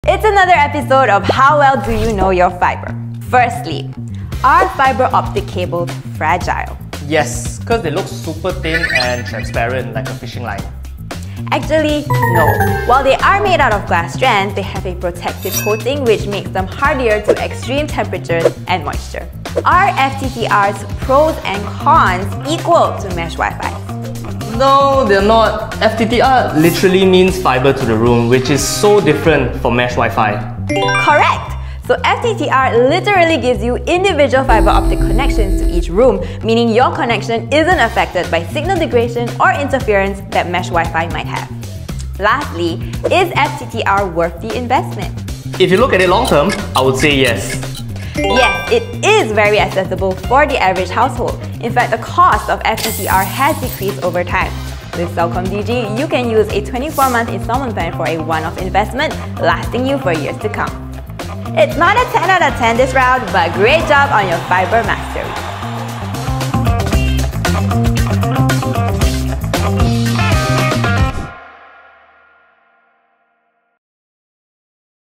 Does your internet take forever to load?
Fibre-To-The Room might be the easy fix to slow and laggy internet, with its maximum speed and coverage in all corners of your home! Here are a few Fibre facts you need to know. 🏠🖥️